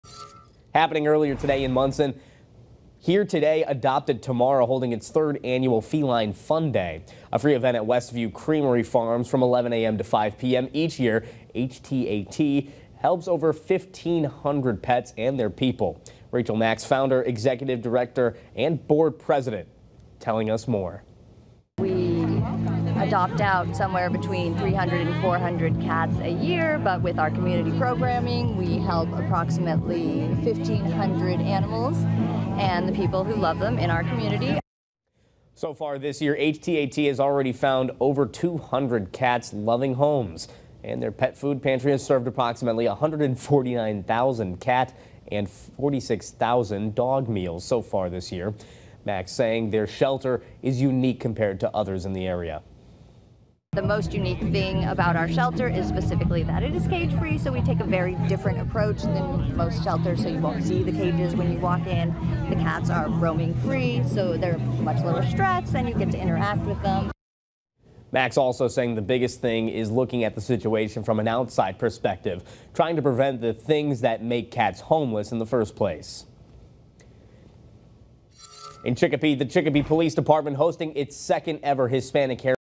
Feline Fun Day held in Monson
‘Here Today Adopted Tomorrow’ hosted its third annual Feline Fun Day on Saturday.